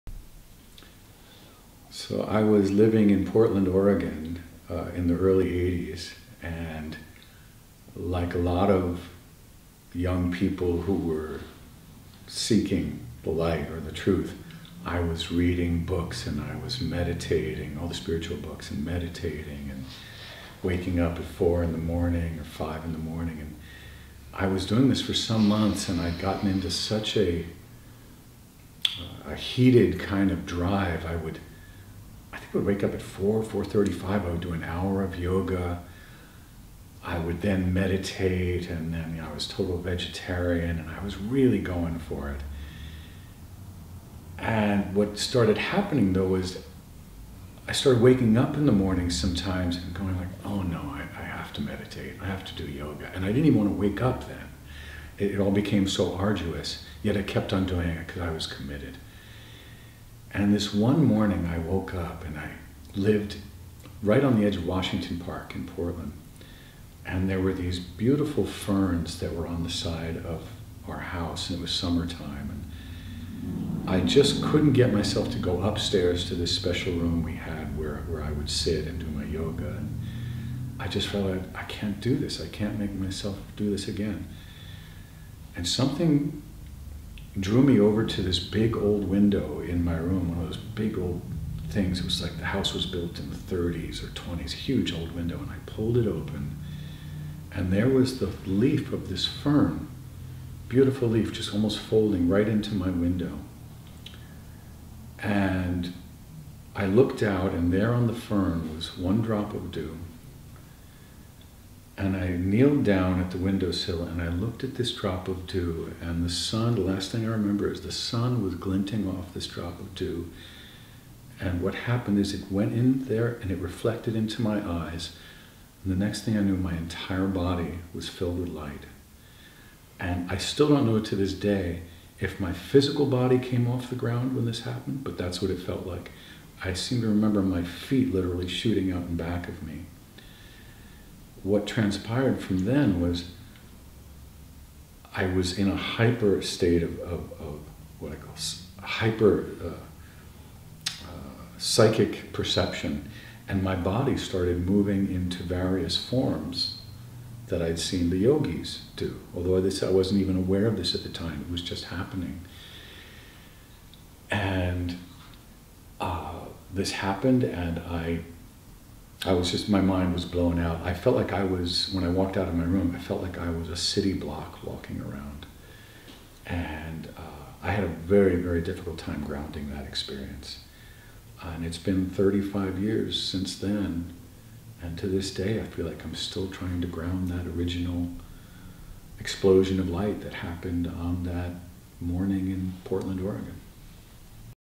Steven Walters, Capitola, CA
Steven abandons his strict spiritual regimen and opens his window to experience a profound shift in reality.